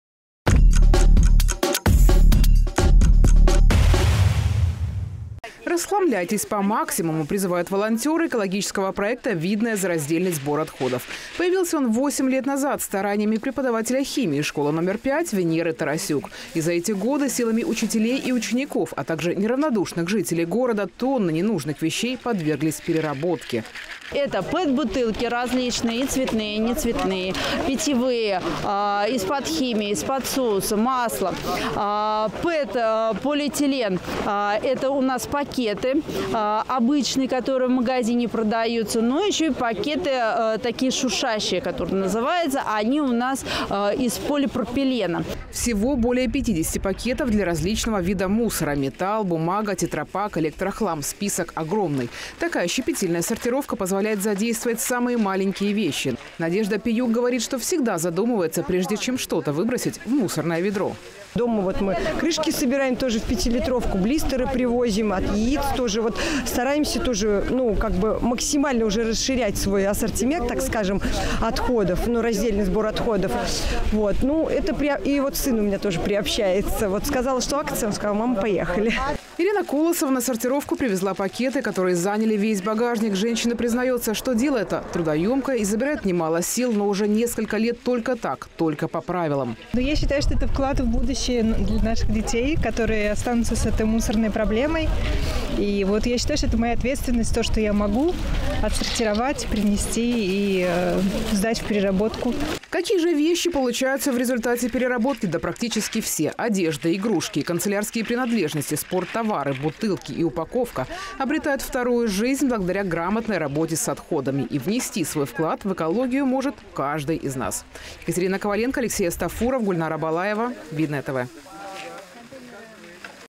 РАЗДЕЛЬНЫЙ СБОР
Раз в месяц в муниципалитете проходят экологические акции проекта "Видное за раздельный сбор отходов". На одной из них побывала наша съёмочная группа